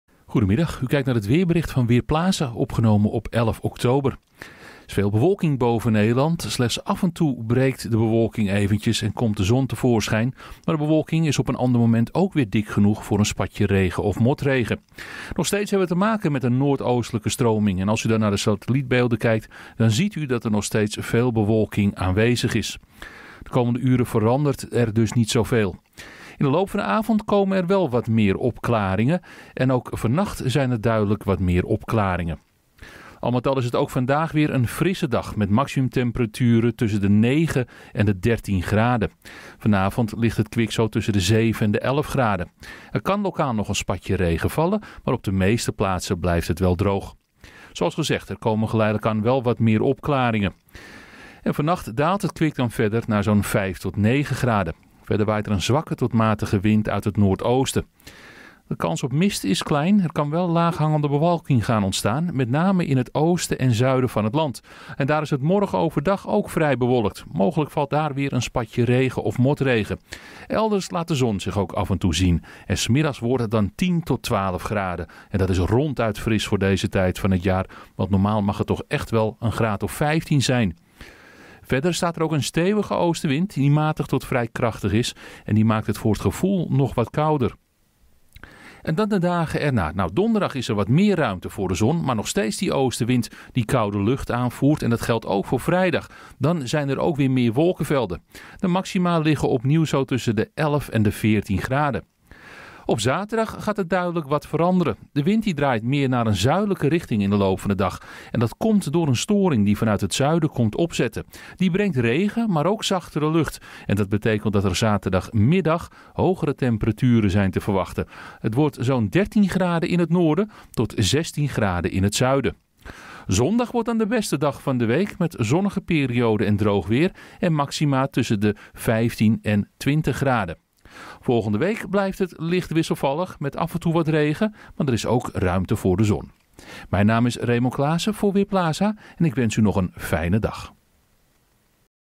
Weerbericht dinsdag: Soms motregen
Er is vrij veel bewolking en er valt soms (mot)regen. In het zuidoosten blijft het waarschijnlijk droog. Het wordt 10 tot 13 graden bij een zwakke tot matige noordoostenwind. Rond de Wadden is de wind (vrij) krachtig, 5 tot 6 Bft. Morgen blijft het op de meeste plaatsen droog en is er soms zon, in het zuiden is er veel bewolking. Het blijft met 10 tot 12 graden fris. Donderdag is er ruimte voor de zon, vrijdag overheerst bewolking. Het blijft fris maar ook droog.